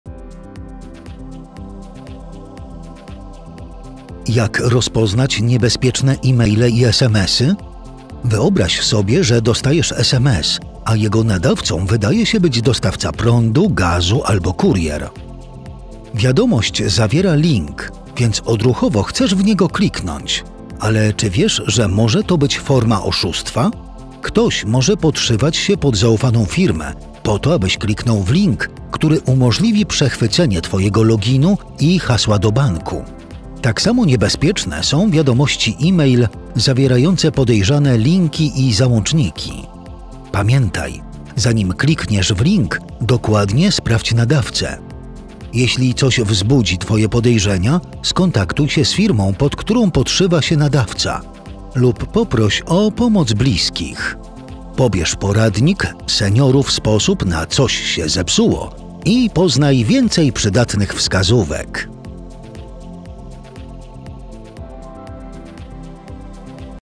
TAURON - Jak rozpoznać fałszywe maile i smsy?
SMS-y czy wiadomości mailowe zawierające szkodliwe linki lub załączniki – to jedna z coraz bardziej popularnych form cyberprzestępstwa. Jak rozpoznać fałszywe maile i SMS-y, co zrobić gdy taka wiadomość budzi wątpliwości?

Zobacz materiał wideo i poznaj odpowiedzi na te i inne pytania.